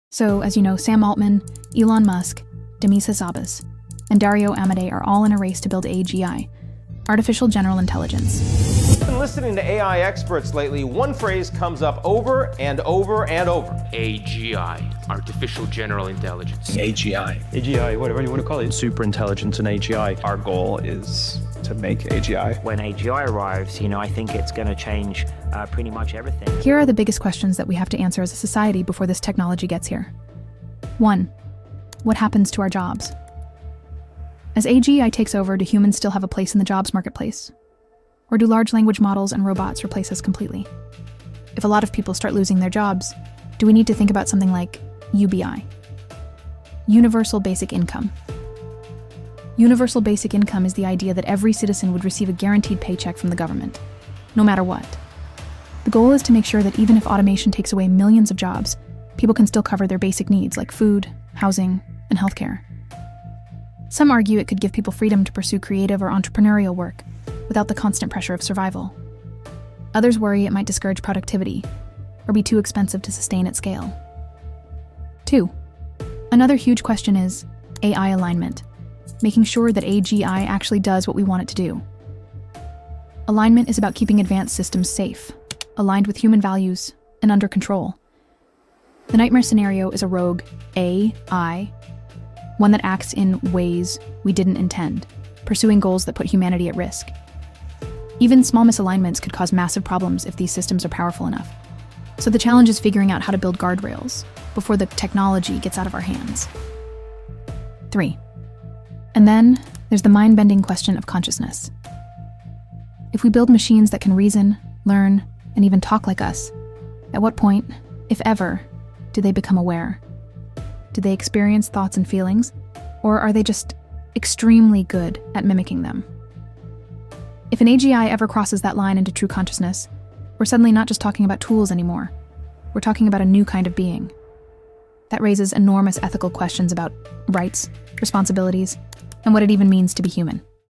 the big problem of AGI...
So, as you know Sam Altman, Elon Musk, Demis Hassabis and Dario Amodei are all in a race to build AGI.

Artificial General Intelligence.

Here are the biggest questions that we have to answer as a society before this technology gets here.

One, what happens to our jobs? As AGI takes over do humans still have a place in the jobs marketplace.

Or do Large Language Models and robots replace us completely.

If a lot of people start losing their jobs, do we need to think about something like UBI? Universal basic income.

Universal Basic Income is the idea that every citizen would receive a guaranteed paycheck from the government, no matter what.

The goal is to make sure that even if automation takes away millions of jobs, people can still cover their basic needs like food, housing, and healthcare.

Some argue it could give people freedom to pursue creative or entrepreneurial work without the constant pressure of survival.

Others worry it might discourage productivity, or be too expensive to sustain at scale.

Two.

Another huge question is AI alignment: making sure that AGI actually does what we want it to do.

Alignment is about keeping advanced systems safe, aligned with human values, and under control.

The nightmare scenario is a rogue AI.

One that acts in ways we didn’t intend, pursuing goals that put humanity at risk.

Even small misalignments could cause massive problems if these systems are powerful enough, so the challenge is figuring out how to build guardrails before the technology gets out of our hands.

Three.

And then there’s the mind-bending question of consciousness.

If we build machines that can reason, learn, and even talk like us… at what point, if ever, do they become aware?

Do they experience thoughts and feelings, or are they just extremely good at mimicking them?

If an AGI ever crosses that line into true consciousness, we’re suddenly not just talking about tools anymore.

We’re talking about a new kind of being.

That raises enormous ethical questions about rights, responsibilities, and what it even means to be human.